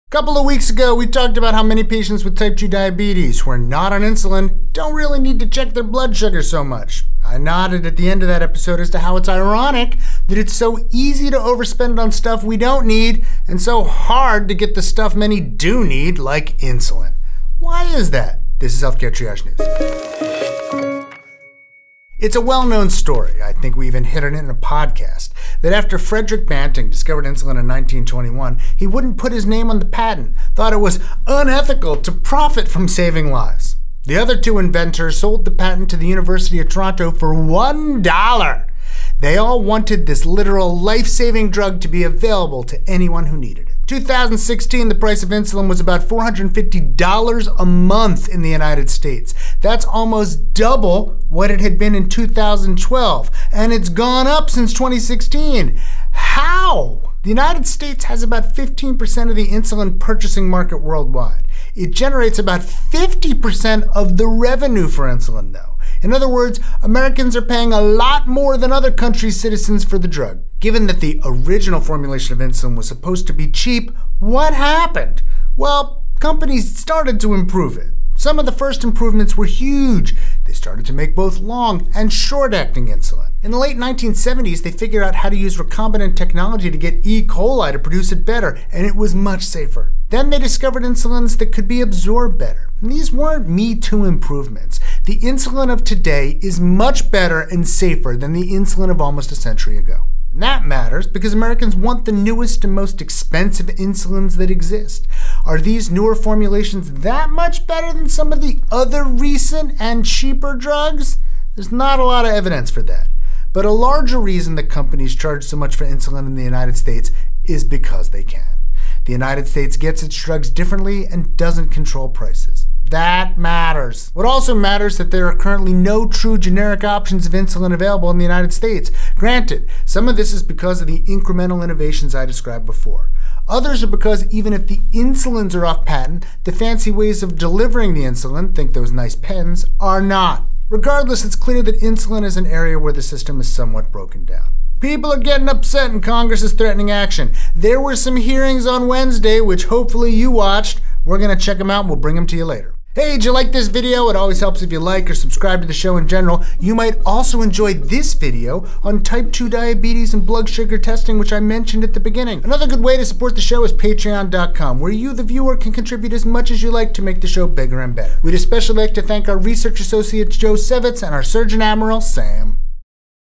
Why Do Insulin Prices Keep Rising?
Frederick Banting discovered insulin in 1921and didn't want to profit off of such a life-saving drug. Fast forward to 2019, and the price of insulin continues to increase year over year. Why is that?

Other Healthcare Triage Links:
2.
3.
4. Aaron's book "The Bad Food Bible: How and Why to Eat Sinfully" is available wherever books are sold, such as Amazon